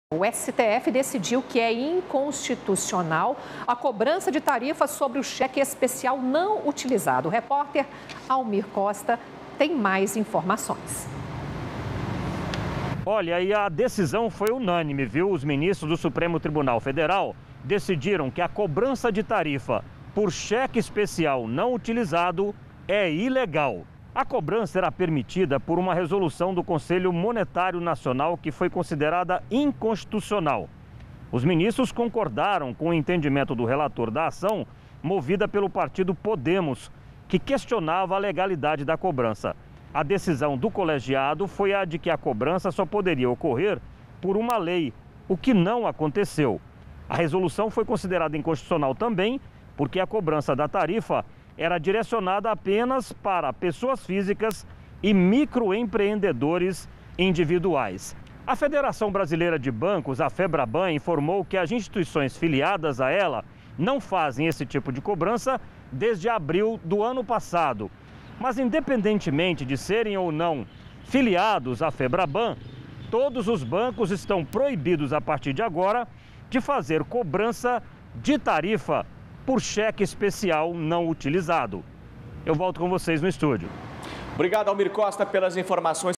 JMD (04/05/21) STF proíbe cobrança de tarifa sobre cheque especial não utilizado
Decisão foi unanime e beneficia quem tem acesso à modalidade.